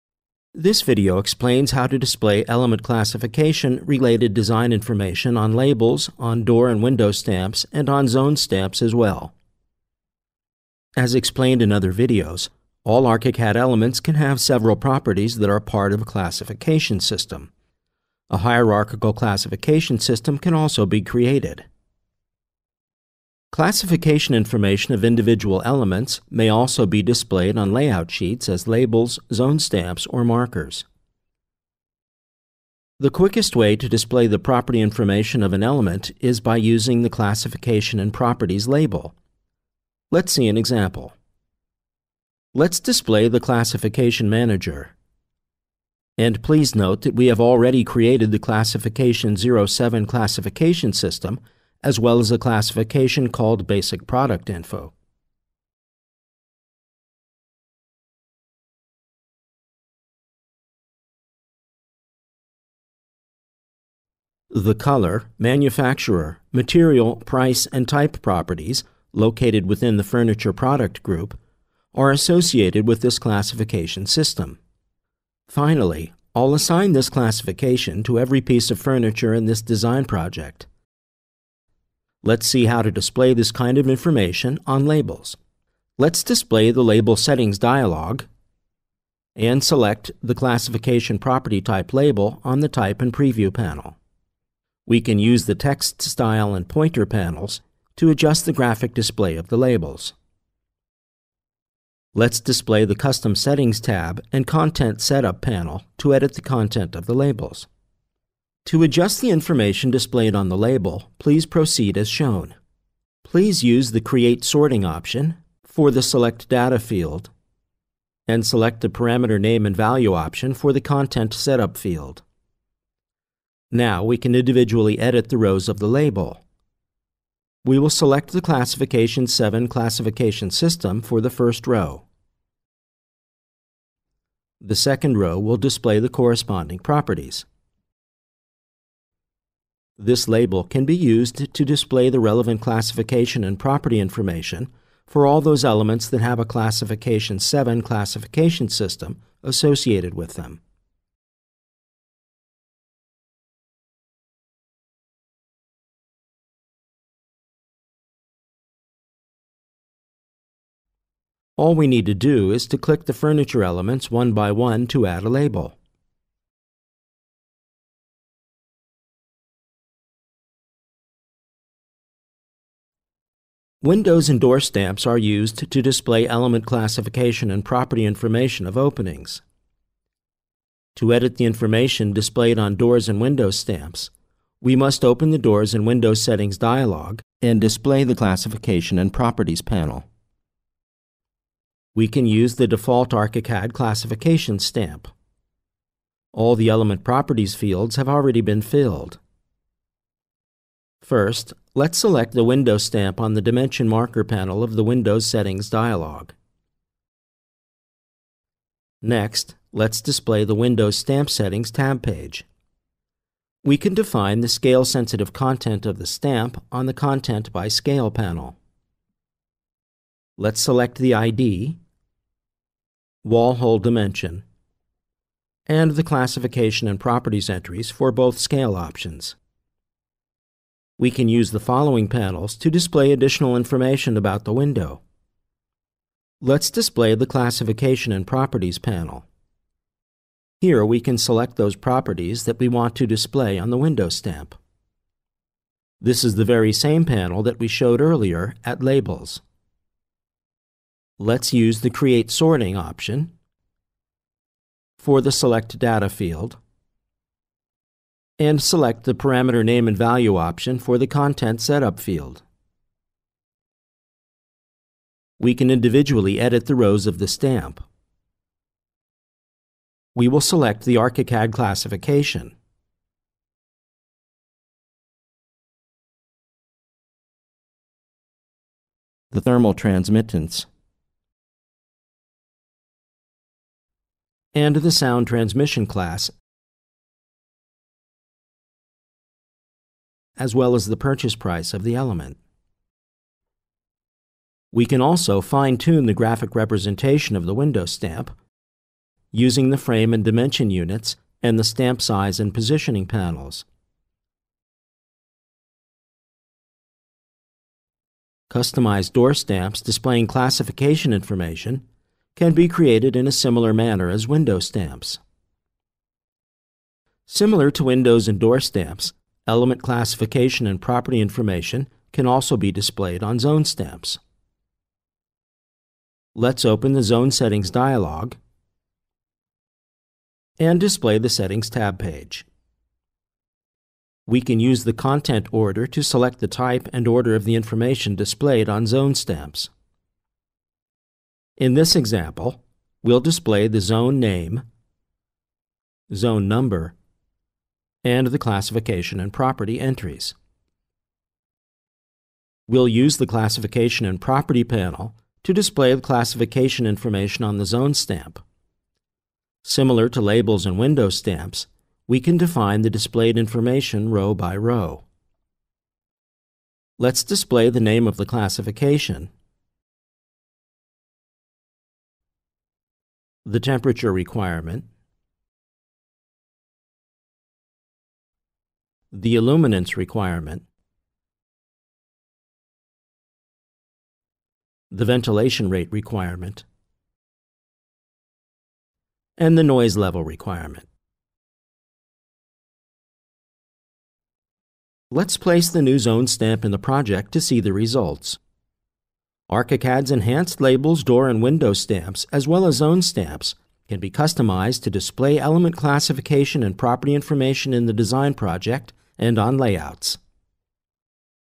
ARCHICAD 21 - Element Classifications: Labels, Window and Door Stamps, Zone Stamps Enhancements
This video explains how to display element classification related design information on Labels, on Door and Window Stamps and on Zone Stamps well. ARCHICAD's enhanced Labels, Door and Window Stamps as well as Zone Stamps can be customized to display element classification and property information in the design project and on layouts.